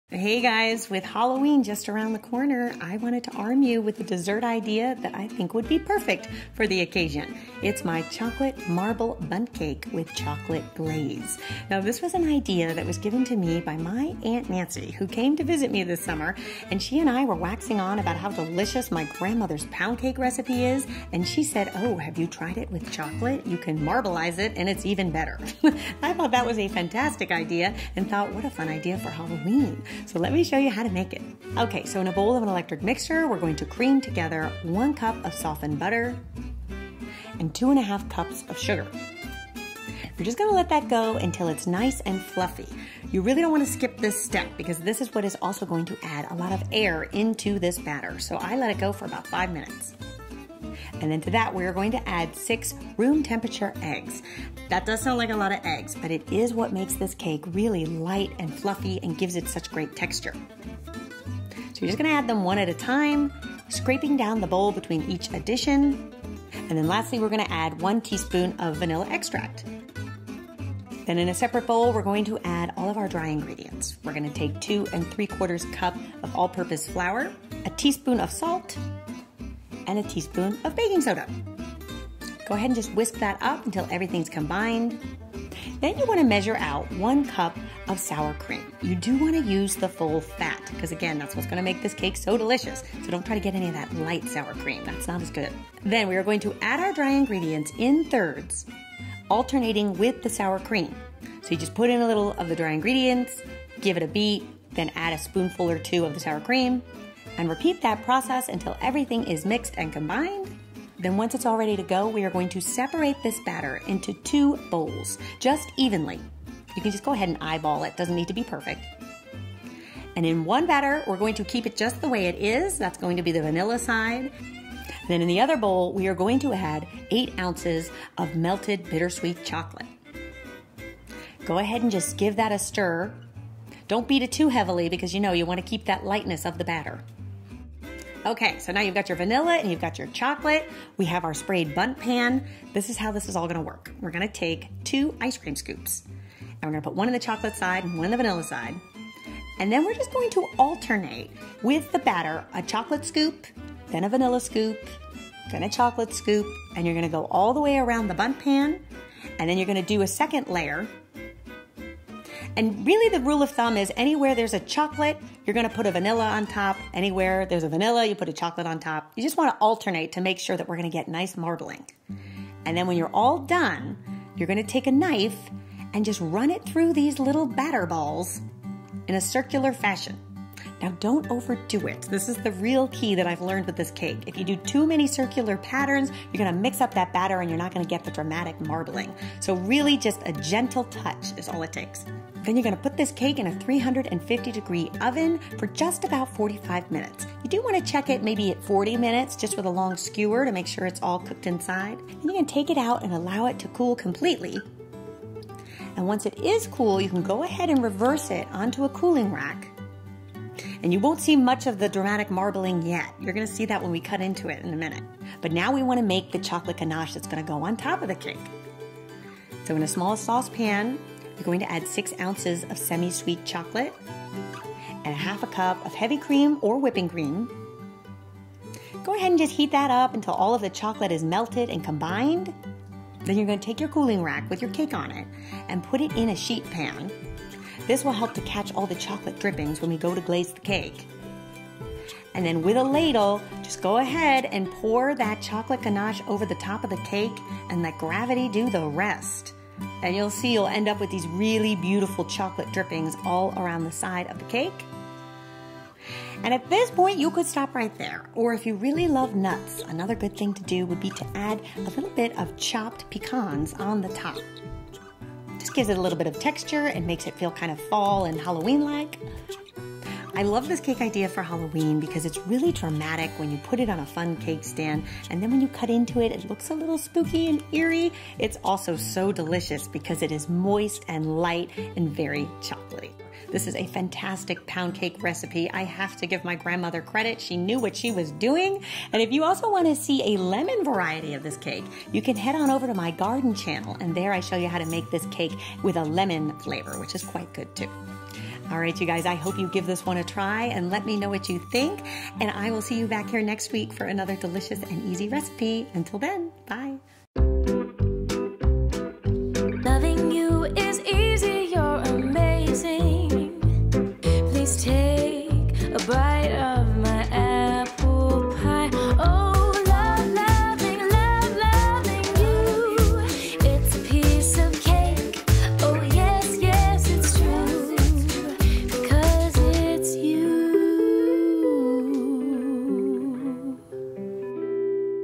Beth's Chocolate Marble Cake Recipe | ENTERTAIINING WITH BETH
Learn how to make a chocolate marble cake an easy Halloween dessert idea!

FOLLOW ME ON PERISCOPE! Entertaining with Beth

BETH’S CHOCOLATE MARBLE POUND CAKE RECIPE
Makes 1 Bundt Cake (serves 12)

INGREDIENTS:
1 cup (240g) butter, softened
2 ½ cups (500 g) sugar
6 eggs, room temperature
1 tsp (5 ml) vanilla
2 ¾ cups (330 g) sifted flour
1 tsp (5ml) salt
1 tsp (5 ml) baking soda
1 cup sour cream (240 ml), room temperature
8 oz (230 g) bitter-sweet chocolate, melted

FOR GLAZE:
6 oz (169 g) semi-sweet chocolate
½ cup (120 ml) heavy cream
2 tbsp (30 ml) chopped pecans

METHOD:
Preheat oven to 350F(176C) degrees.

Spray Bundt pan with baking spray.

Whisk together the flour, salt and baking soda and set aside.

Cream together butter and sugar until very light and fluffy.About 5-7 mins.

Add the eggs to the butter mixture, 1 at a time.  For a quick way to warm up eggs to room temperature, place them in a bowl with warm water for 5 mins while butter is creaming together).

Add the eggs 1 at a time, scrapping down the bowl as needed, so that the eggs are well incorporated.

Then add the flour mixture to the batter in thirds, alternating with a spoonful of the sour cream. just until combined, being careful not to over mix! (or your cake will be tough)

Separate half of the batter and put it in a separate bowl.  To that stir in the melted chocolate until combined.

Spoon in batter with an ice cream scoop alternating plain and chocolate scoops all the way around the Bundt pan, creating a second layer on top, also alternating flavors.

Run a knife through the batter balls in a circular fashion, creating a marble design. Be careful not to "over swirl" or you'll mix the batter too much and you won't get good marbling.

Smooth out the batter on top of the cake once swirling is done.

Bake for 45-50 mins.  It’s done when a toothpick comes out clean. Allow to cool completely.  Reverse from mold and place on a sheet pan, fitted with a cooling rack.

GANACHE:
Place chocolate and cream in a small sauce pan, heat until combined and smooth. If you feel you need to thin the chocolate a bit in order to drizzle better just add 1 tsp of vegetable oil until consistency is ideal.

Ladle melted chocolate over cooled cake, top with chopped pecans and allow to set.

Serve with vanilla ice cream or homemade whipped cream!  Enjoy! :)

Hi! I'm Beth Le Manach and I believe food tastes better when shared. New Videos post every Saturday!